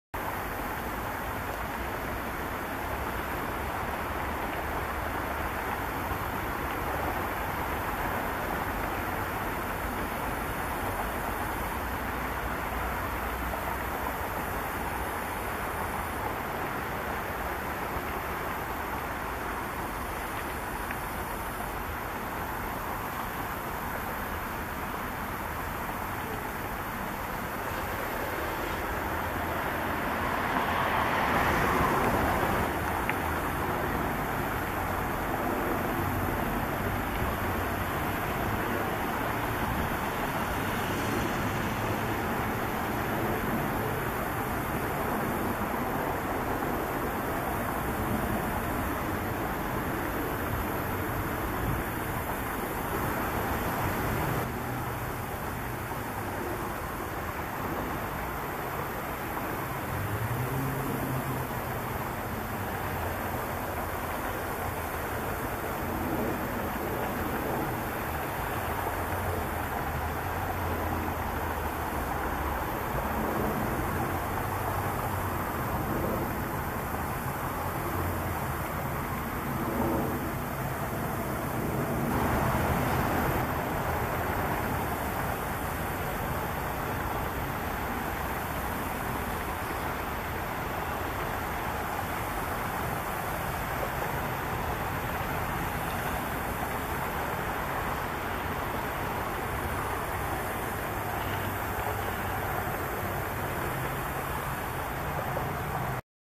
SnowyEgret - Fishing
A Snowy Egret fishing in a small puddle near a creek in Carrollton, Texas.